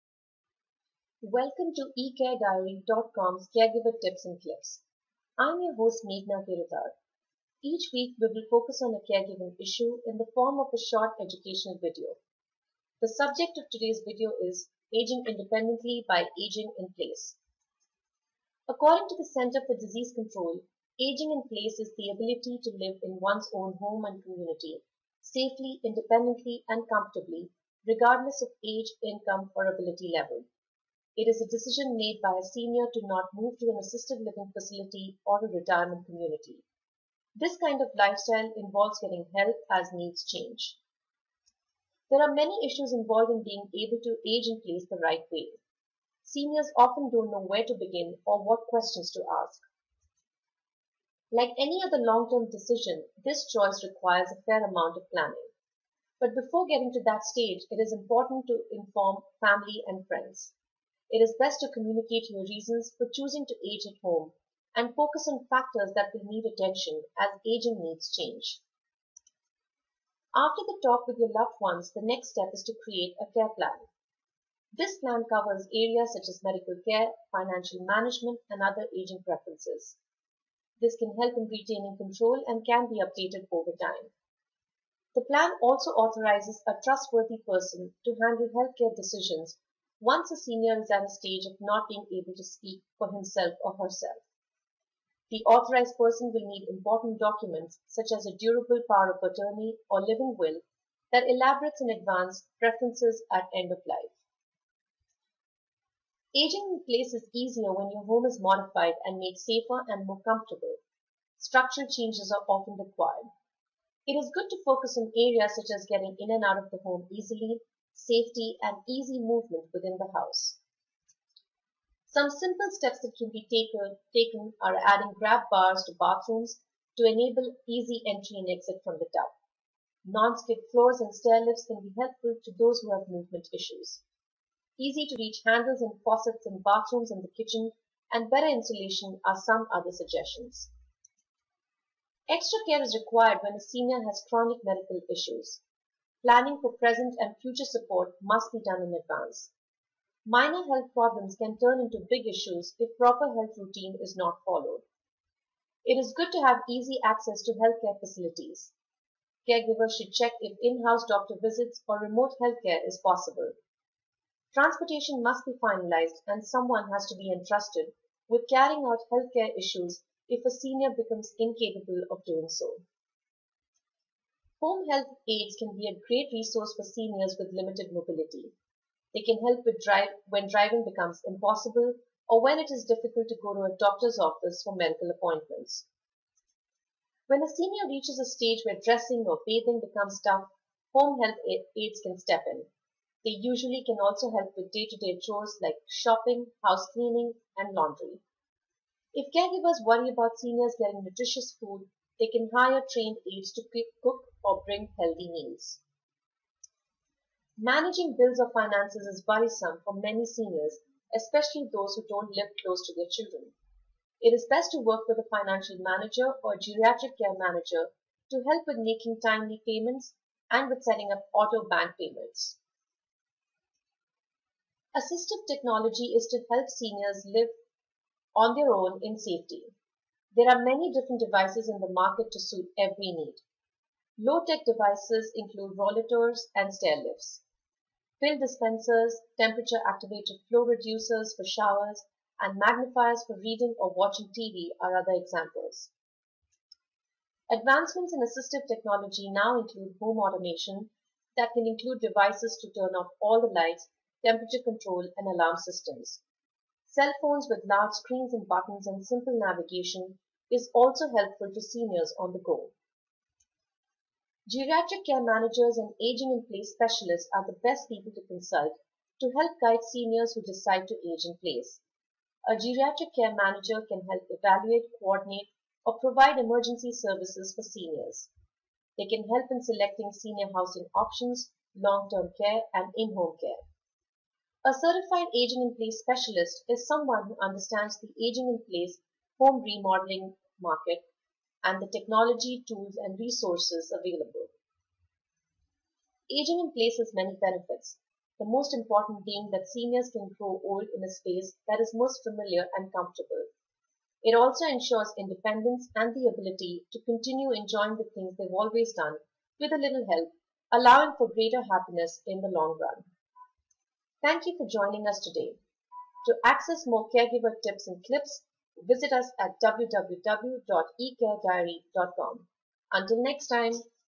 Aging Independently by Aging in Place
Aging in place is the ability for a senior to live in his or her own home, safely, independently and comfortably. This video highlights steps and areas to ensure a successful aging in place plan.